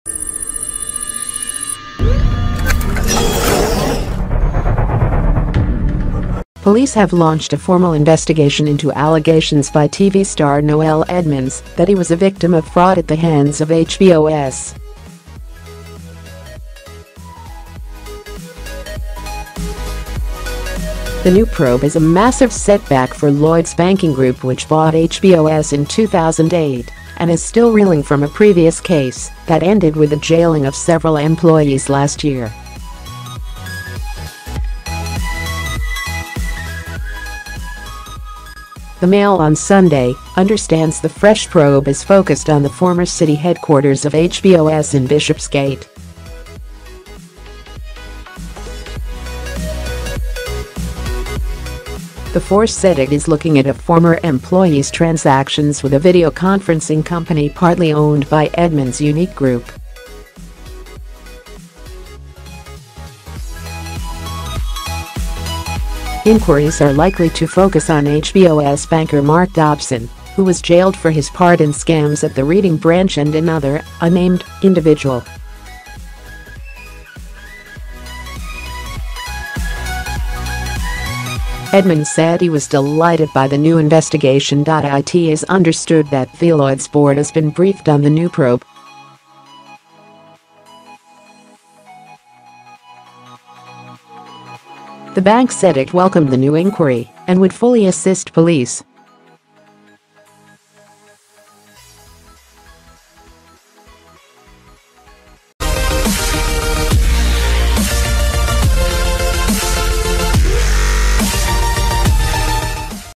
Noel Edmonds wins new fraud inquiry into HBOS - News Techcology
Police have launched a formal investigation into allegations by TV star Noel Edmonds that he was a victim of fraud at the hands of HBOS.The new probe is a massive setback for Lloyds Banking Group which bought HBOS in 2008 and is still reeling from a previous case that ended with the jailing of several employees last year.The Mail on Sunday understands the fresh probe is focused on the former City headquarters of HBOS in Bishopsgate.The force said it is looking at a former employee's transactions with a video-conferencing company partly-owned by Edmonds' Unique Group.Inquiries are likely to focus on HBOS banker Mark Dobson, who was jailed for his part in scams at the Reading branch and another, unnamed, individual.Edmonds said he was 'delighted' by the new investigation. It is understood that theLloyds board has been briefed on the new probe.The bank said it welcomed the new inquiry and would fully assist police.